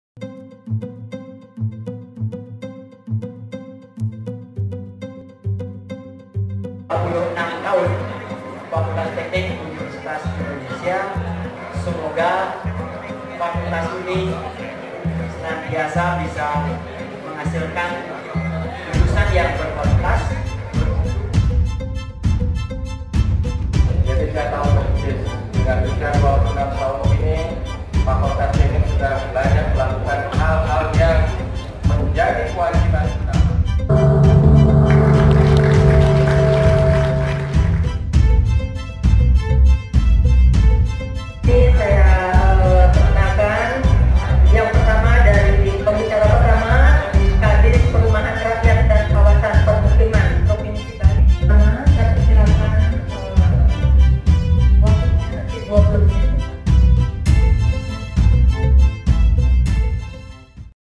26 Tahun Fakultas Teknik UNHI - Sarasehan, Launching buku dan penandatanganan MOU
agenda 'Sarasehan Permukiman Layak Huni dan Berkelanjutan' dalam rangka 26 Tahun Fakultas Teknik UNHI, sekaligus launching buku Permukiman yang Harmonis dari tim dosen Fakultas Teknik UNHI dan penandatanganan MOU UNHI dengan Intakindo

Sekaligus pelantikan BEMFT, HMTS dan HMPWK UNHI